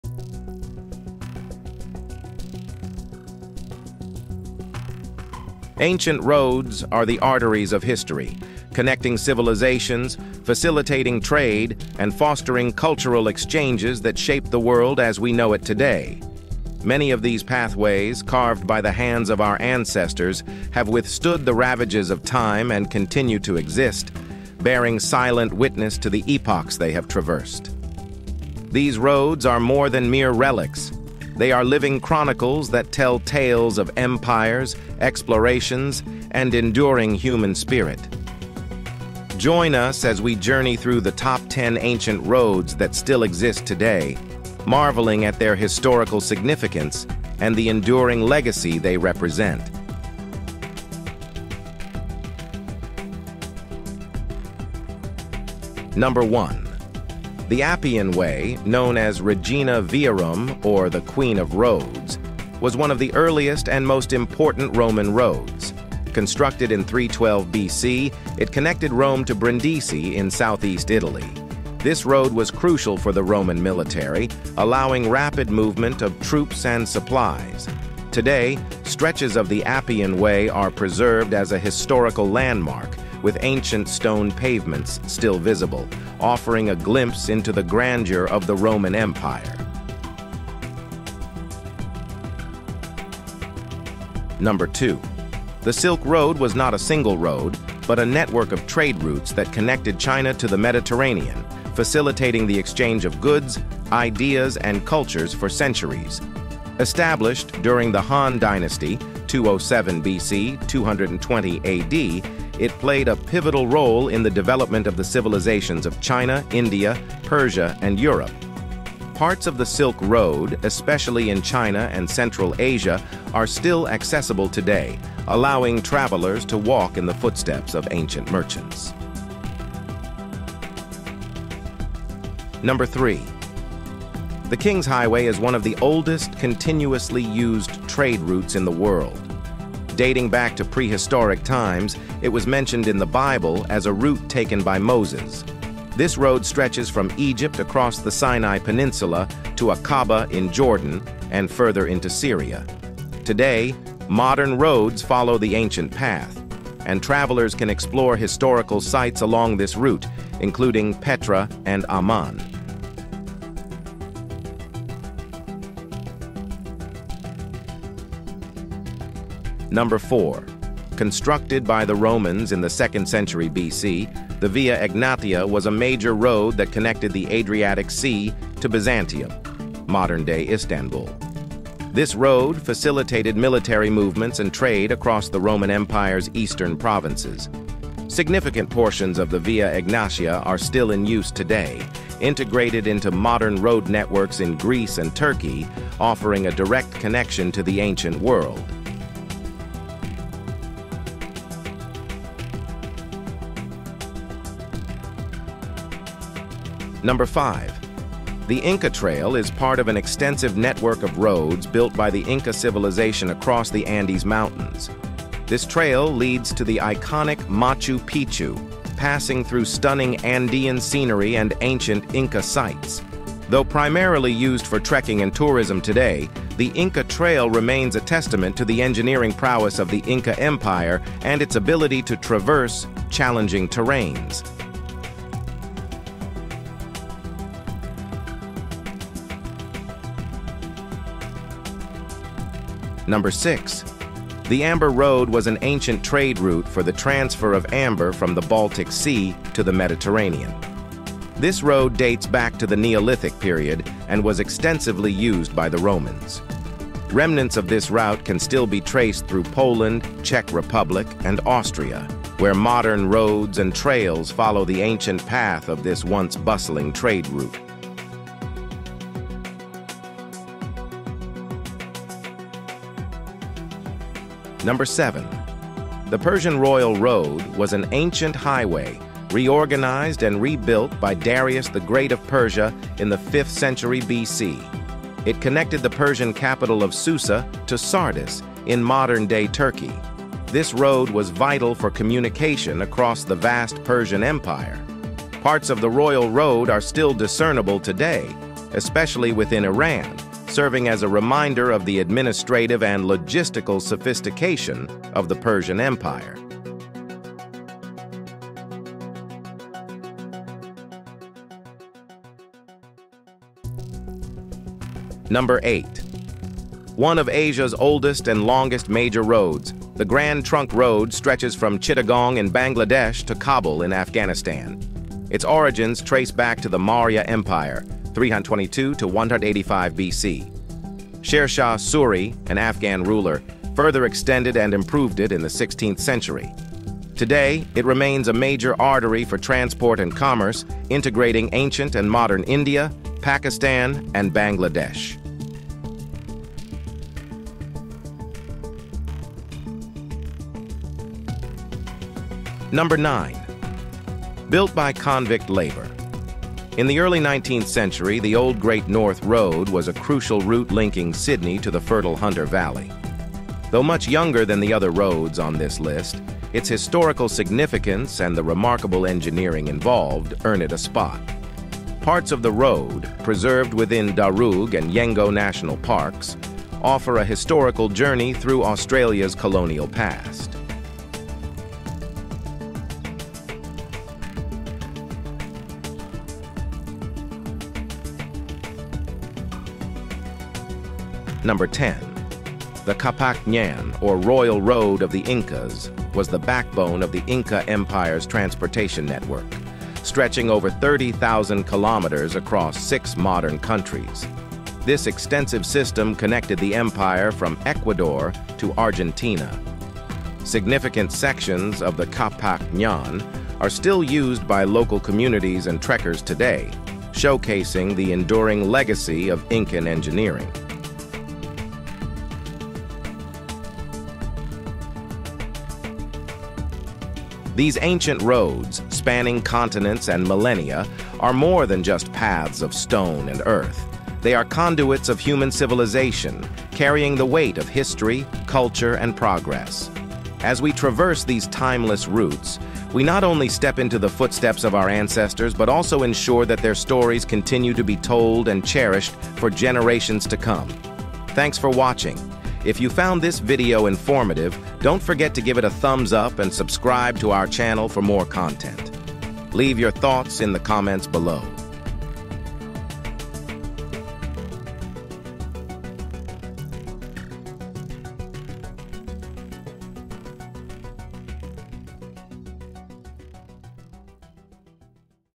TOP 10 ANCIENT ROADS THAT STILL EXIST TODAY
These roads are more than mere relics; they are living chronicles that tell tales of empires, explorations, and enduring human spirit. Join us as we journey through the top 10 ancient roads that still exist today, marveling at their historical significance and the enduring legacy they represent.